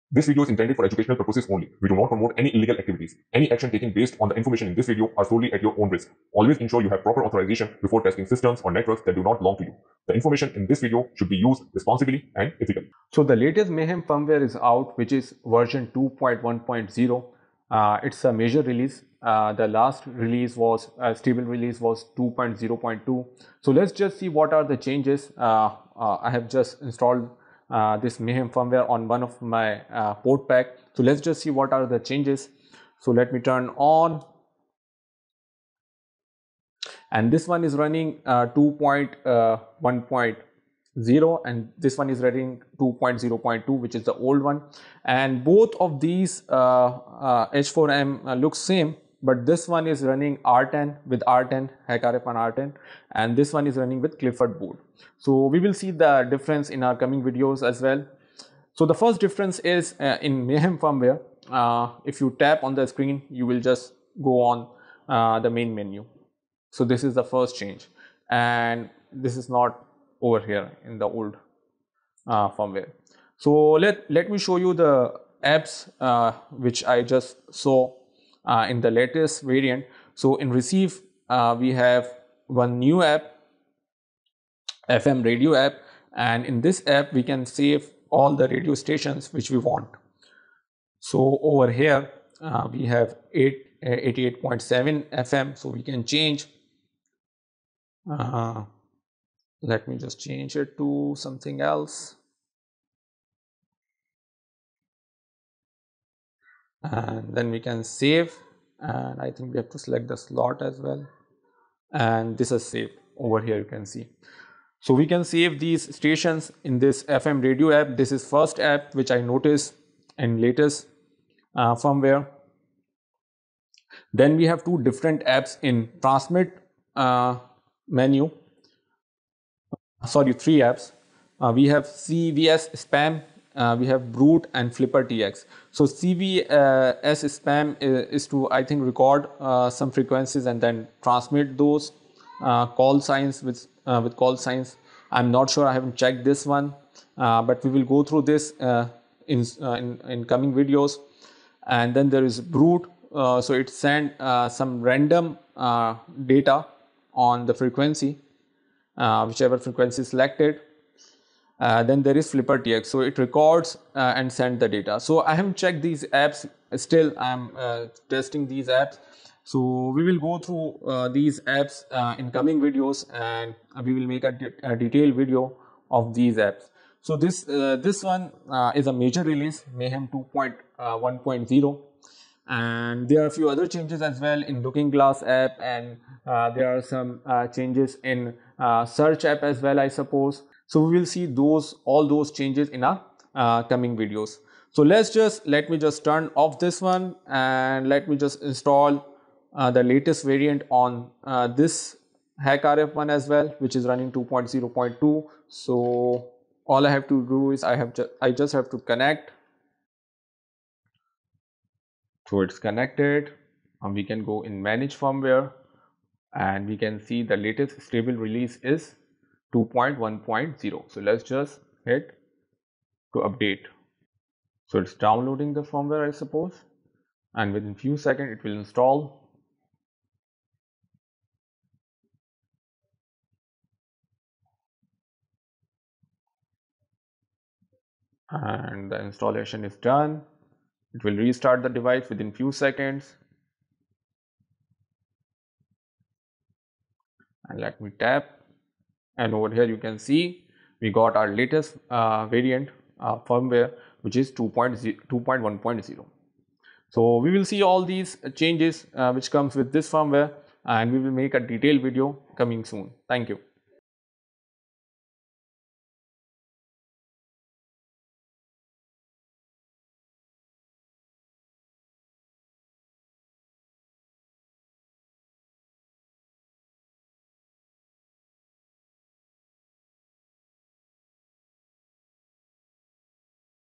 Upgrade to Mayhem 2.1.0: New Features & Update Steps for HackRF One PortaPack
Discover the latest Mayhem 2.1.0 firmware for HackRF One PortaPack! In this video, we’ll guide you through:

Updating your PortaPack from Mayhem 2.0.2 to 2.1.0.
Exploring the new features, improvements, and bug fixes in the latest version.